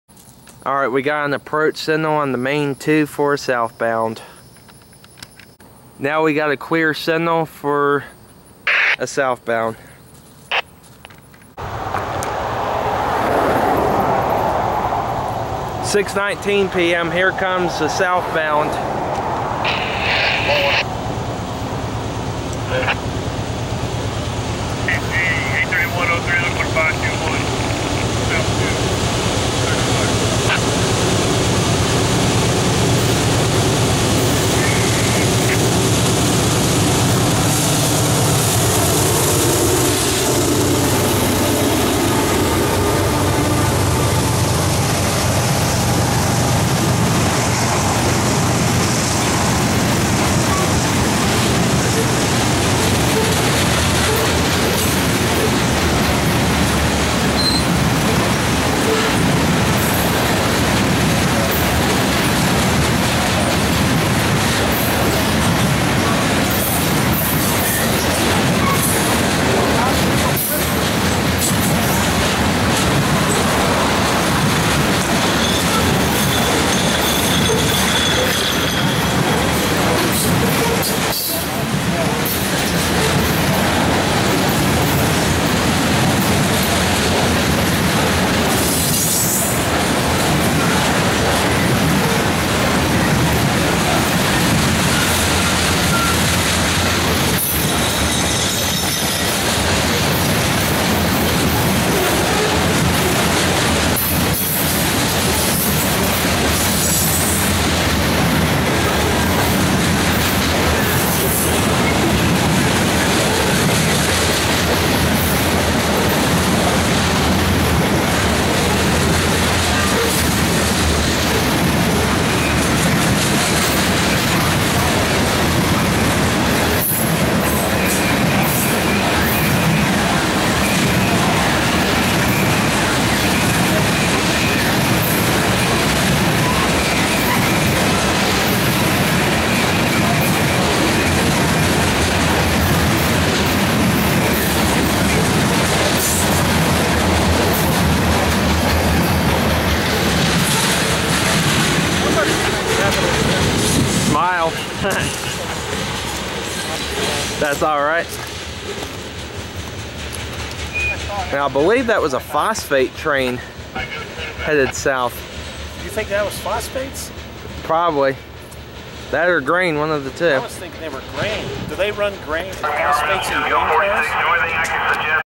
CSXT G831
6:19 PM

Unexpected surprise here...G831, a unit loaded grain train, heads south with an AC44CW and an ES44AH for power. *THIS VIDEO IS AVAILABLE IN 1080p60* Shot with a Sony HDR-CX380.

Recorded 7.8.2015